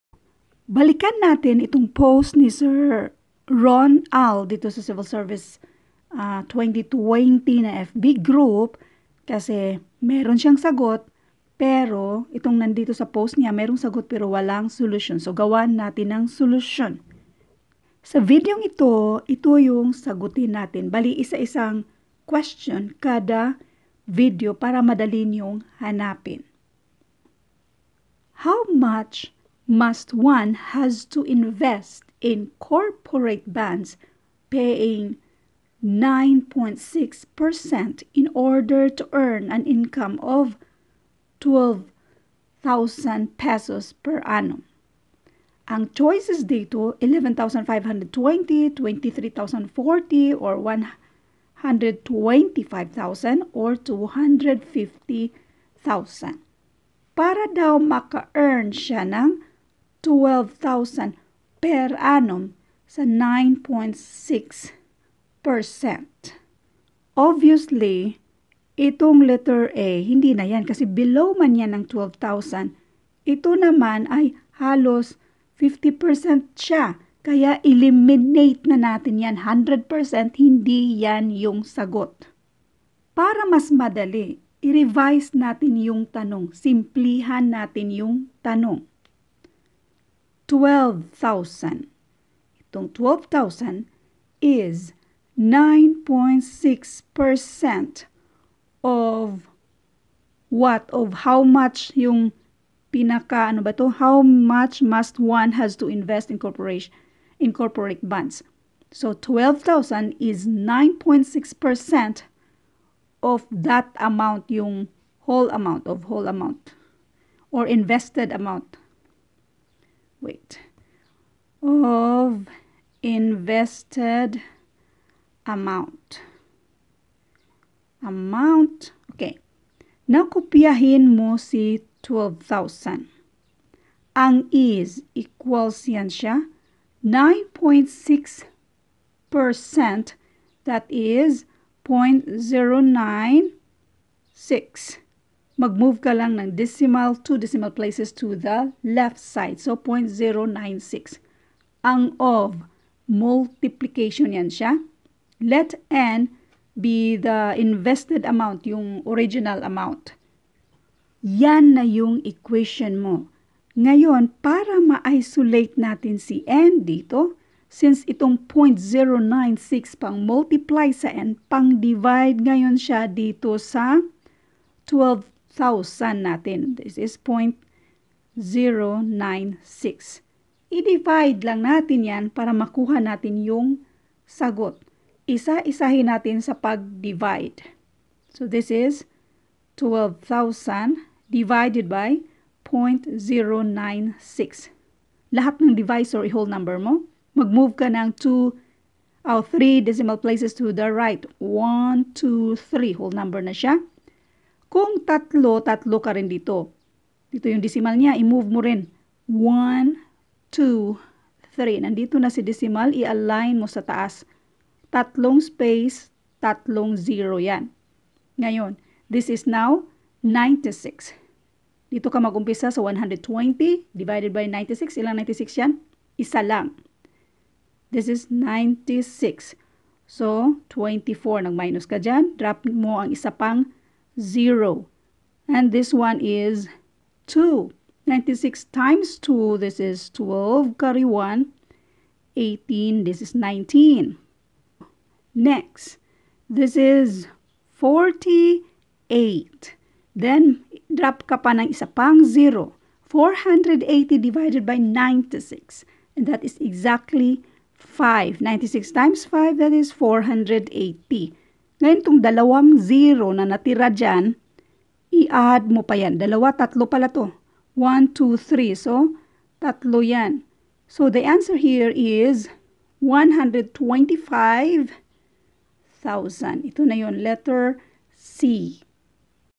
How much must one has to invest in corporate paying 9.6% in order to earn an income of P12,000/yr?
How much must one has to invest in corporate bonds paying 9.6% in order to earn an income of Php12,000 per annum?
a. Php 11,520
b. Php 23,040
c. Php125,000
d. Php 250,000